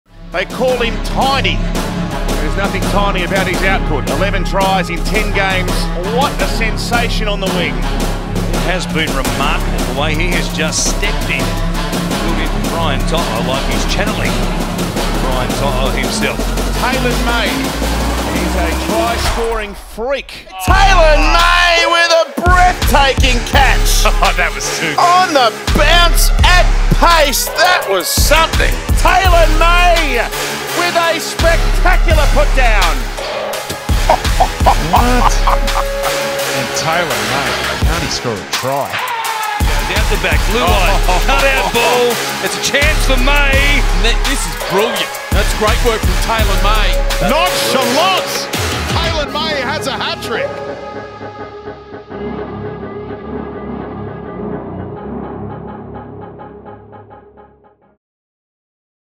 "HE'S A TRY SCORING FREAK!" 🔥 | Taylan May 2022 Season Summary
It was a rookie season to remember for Panthers winger Taylan May.

After 21 NRL games, he was named the Ben Alexander Rookie of the Year for the Panthers, finished as the club's top try-scorer with 16 and won the OAK Plus Try of the Year at the Merv Cartwright Medal.

Sync ID: MB01BMWD6T2DTHI